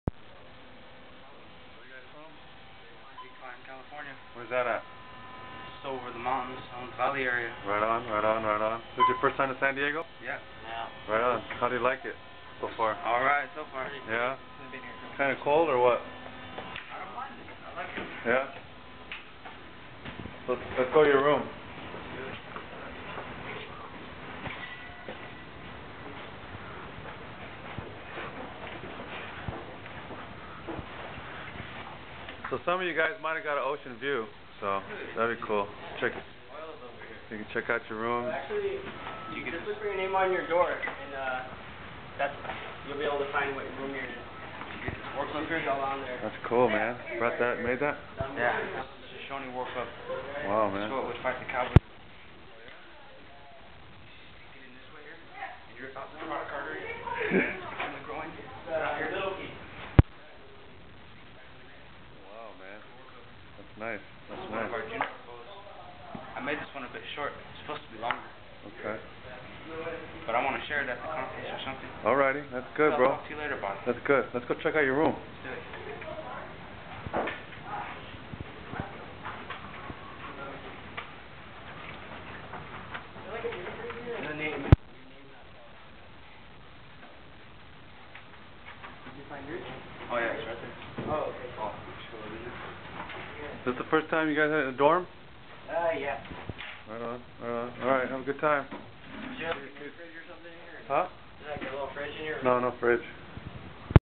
San Diego 2009 Paiute Youth Arrive
Youth from Big Pine arrive to share and represent their people with InterTribal Youth Summer San Diego Program.